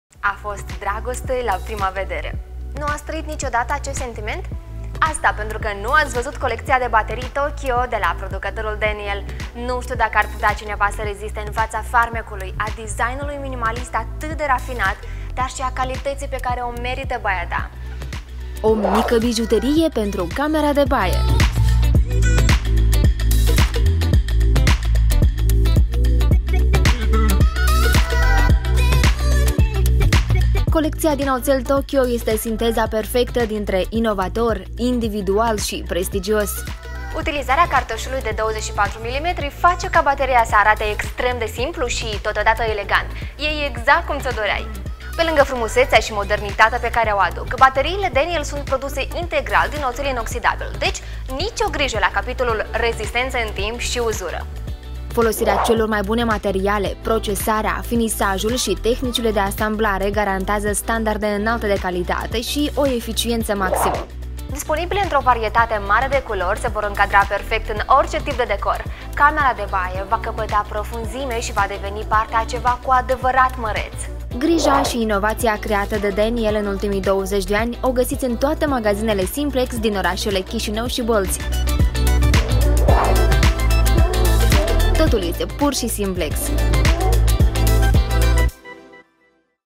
Baterii din colectia Tokyo de la Daniel
Simplex vă prezintă colecția de baterii Tokyo de la producătorul Daniel.
Colecția este sinteza perfectă dintre inovator, individual și prestigios. Utilizarea cartușului de 24 mm face ca bateria să arate extrem de simplu și totodată elegant. E exact cum ți-o doreai.
Folosirea celor mai bune materiale, procesarea, finisajul și tehnicile de asamblare garantează  standarde înalte de calitate și o eficiență maximă.
Disponibile într-o varietate mare de culori, se vor încadra perfect în orice tip de decor. Camera de baie va căpăta profunzime și va deveni parte a ceva cu adevărat măreț.

Grija și inovația creată de Daniel, în ultimii 20 de ani, o găsiți în toate magazinele Simplex din București - România, Chișinău și Bălți - Republica Moldova.